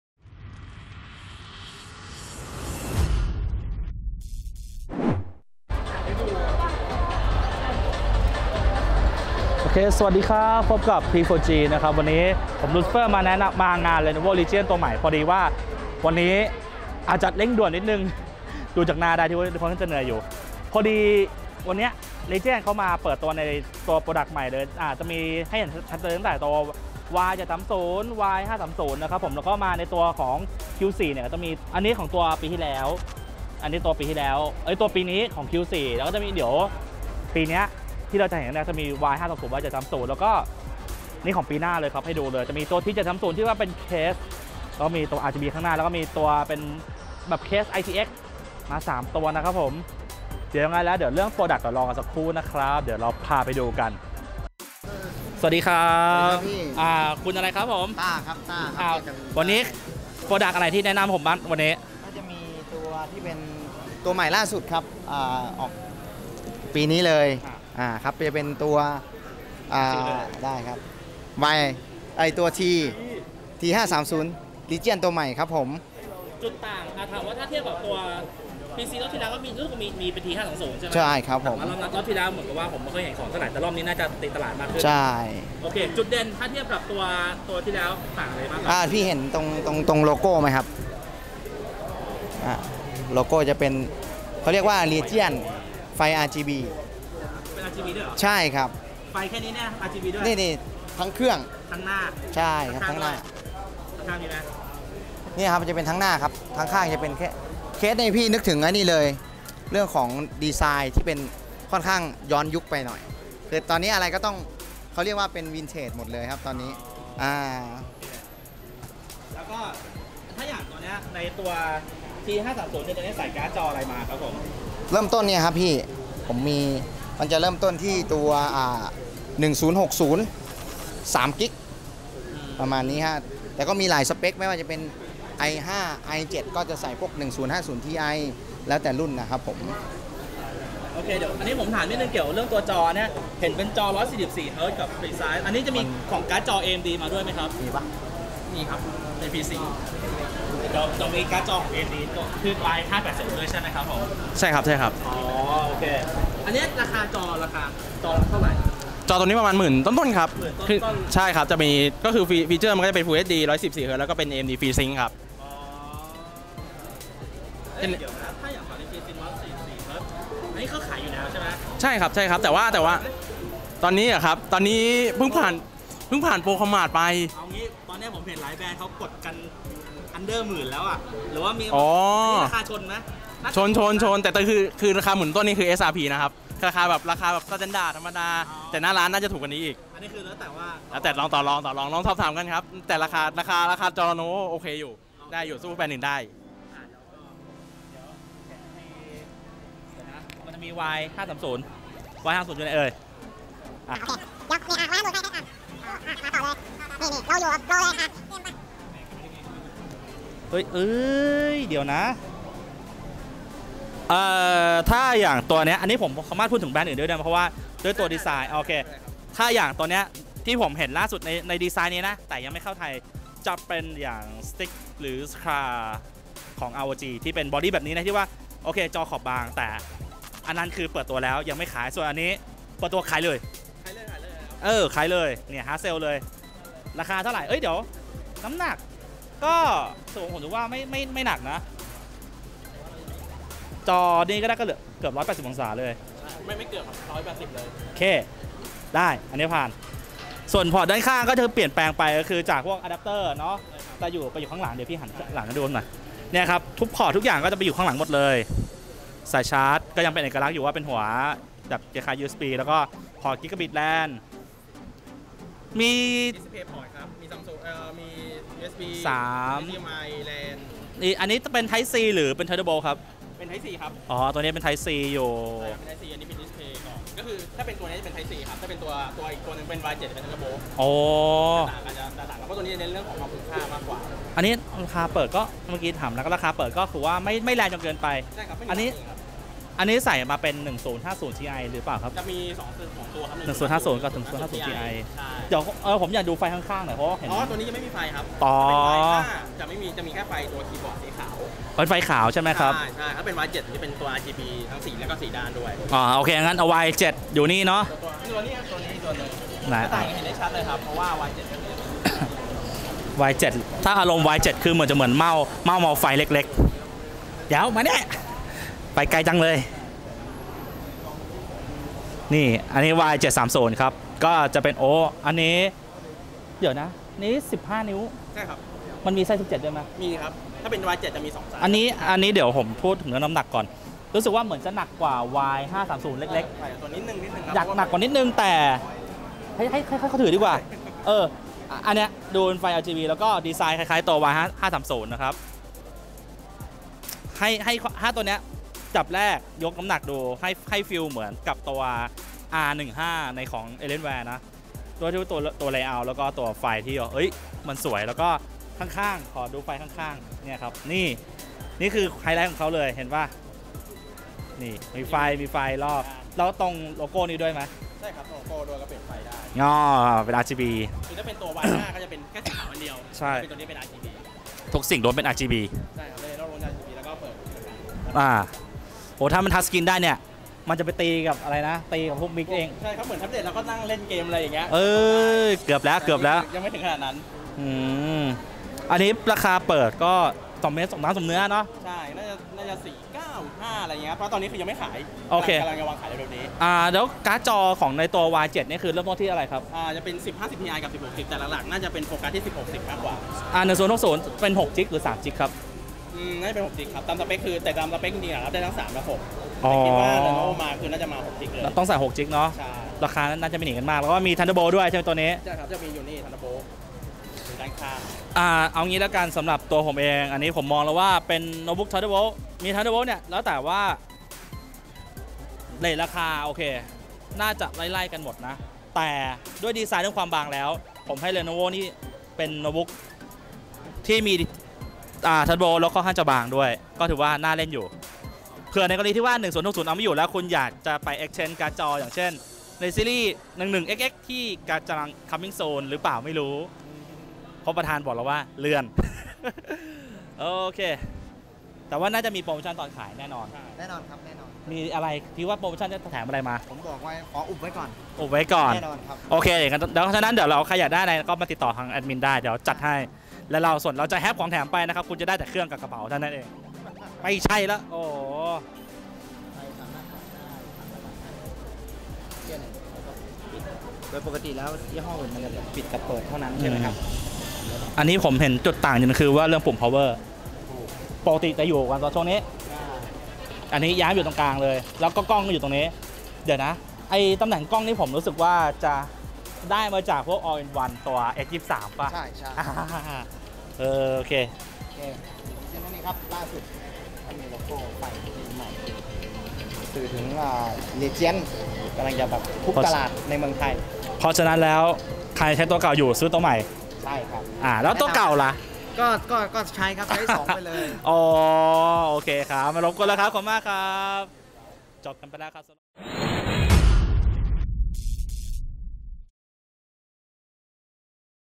(P4G) พาชมผลิตภัณฑ์ใหม่ Lenovo - T530, Yoga 530, Y730 และจอ Y25f-10
เมื่อวันที่ 29 มิถุนายนที่ผ่านมา เลอโลโวได้จัดงานแถลงข่าวเปิดตัวผลิตภัณฑ์ใหม่ทั้งสายเกมและสาย Life Style โดยเราได้มีคลิปแนะนำผลิตภัณฑ์ใหม่มาฝากกัน โดยสำหรับคลิปนี้จะเป็นทางฝั่งของเกมครับ โดยมีจะมีอะไรบ้างนั้น มาชมกันครับ

ผลิตภัณฑ์ที่ปรากฎในคลิป
- Lenovo Legion T530
- Lenovo Legion Y25f-10 Gaming Monitor

สามารถสอบถามรายละเอียด และราคาของผลิตภัณฑ์ได้ที่แฟนเพจของเลอโนโวได้เลยนะครับ